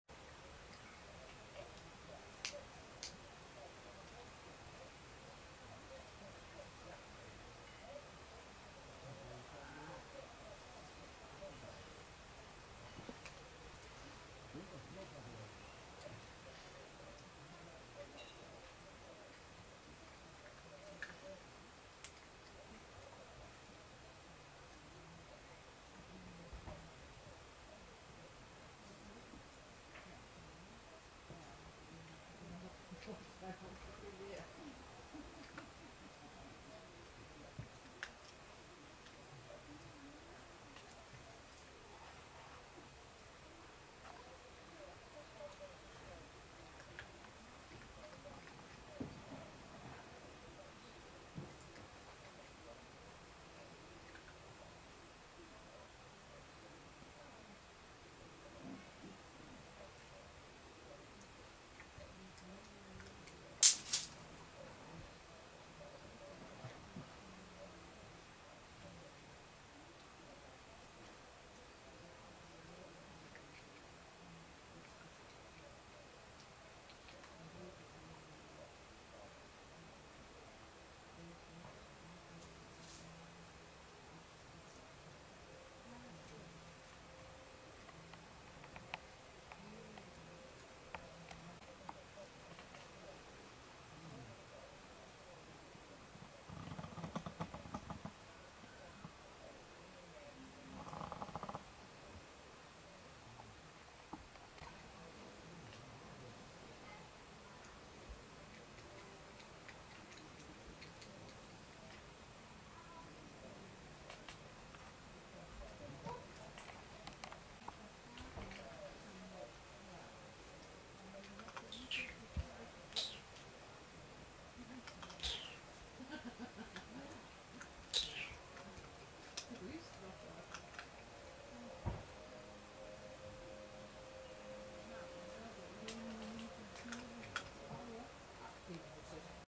Vos op de camping
Vos aan het eten uit de voerbak van de egels op camping de Nollen in Callantsoog.